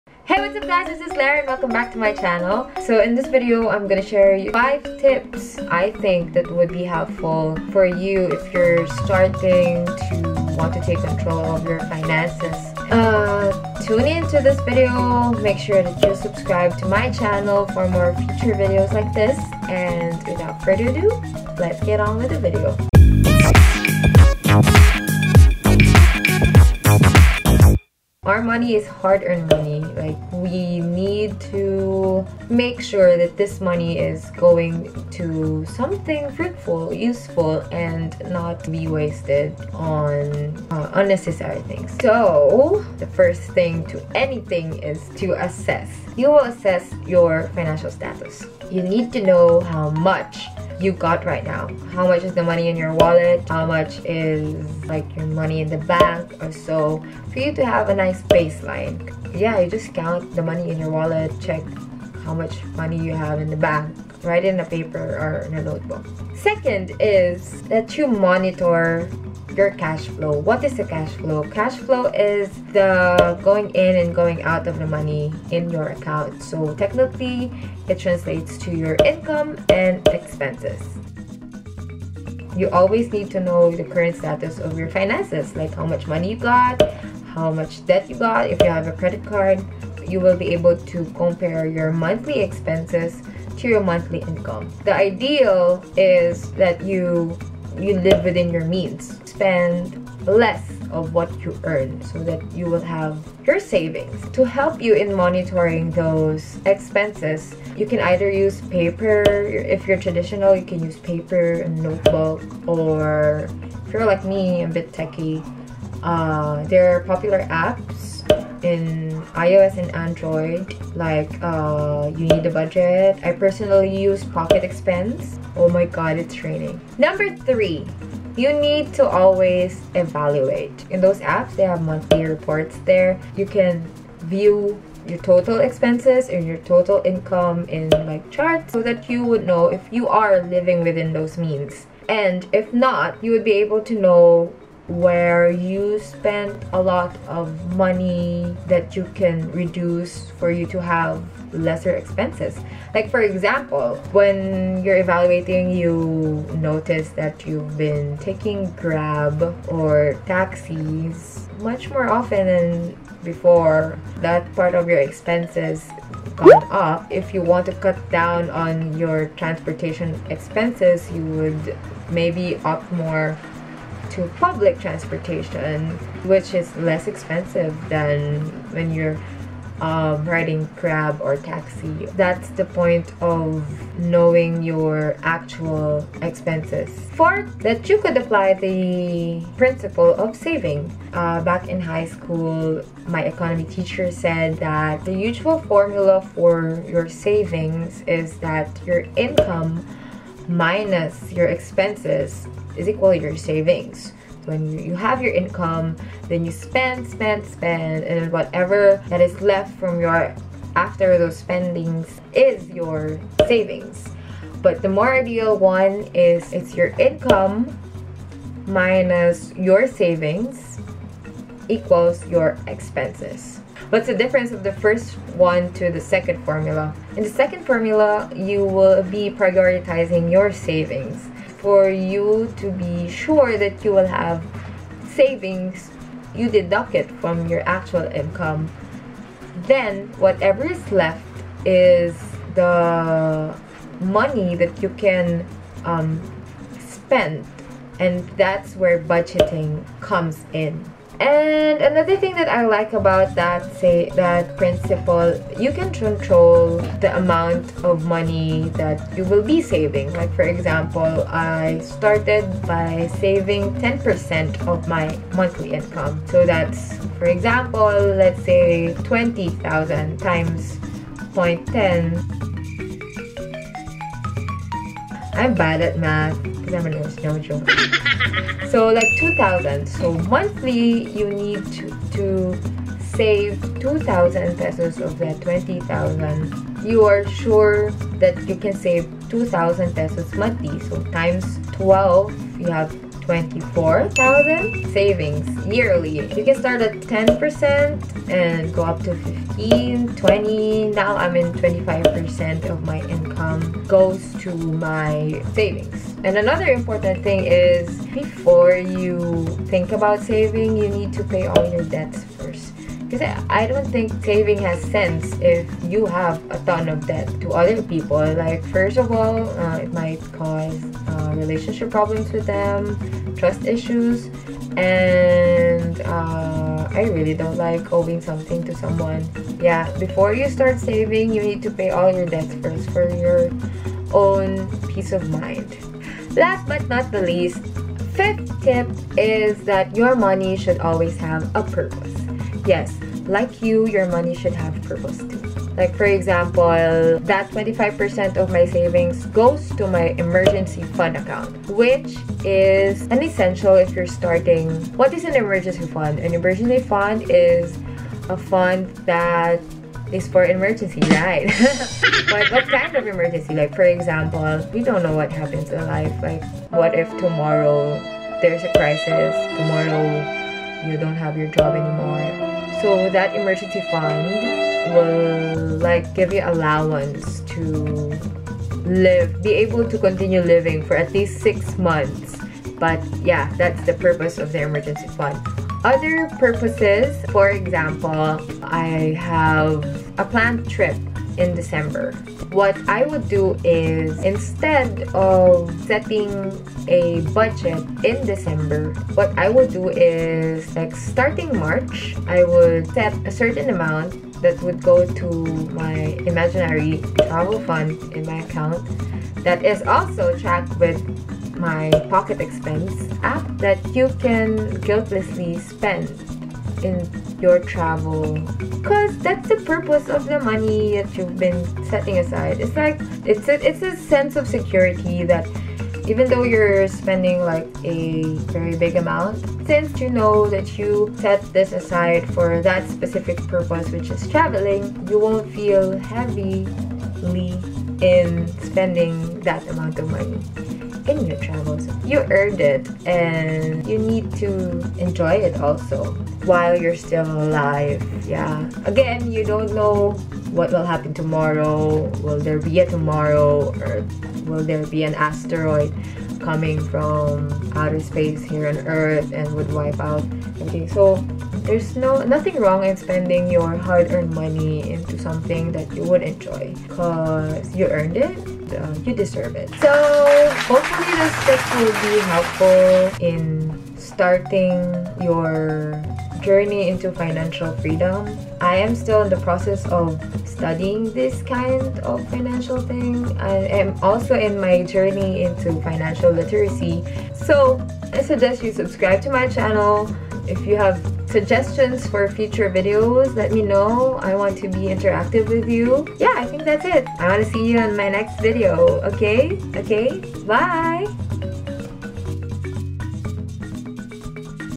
HOW TO SAVE MONEY WHEN YOU START WORKING | FABB LAGAS
Since I'm new to this "adulting" thing, I really tried my best to understand how to manage my money the best way possible. So I'm sharing these five tips I have learned along the way to young adults like me to hopefully inspire them to start controlling their finances too. Happy saving everyone!

Adobe Premiere Pro 2020 | Adobe Photoshop 2020 | Bandicam Screen Recorder